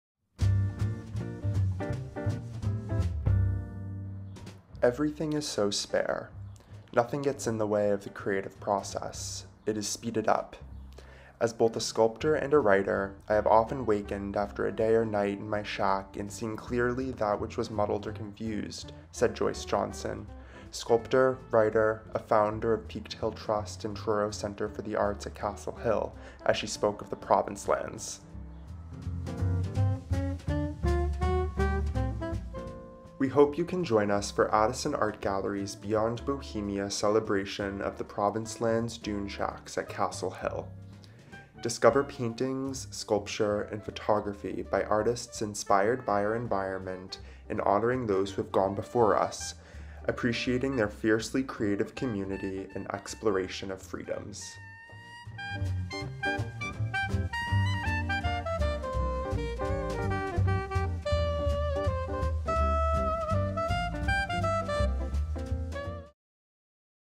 Beyond Bohemia at Truro Center for the Arts at Castle Hill
August 22 through September 1
Reception Thursday, August 24 from 4:00 to 6:00
10 Meetinghouse Road, Truro

Join us for a new exhibition including work by a diversity of contemporary artists highlighting the dune shacks, and works by dune dwellers William S. Fitts and Joyce Johnson.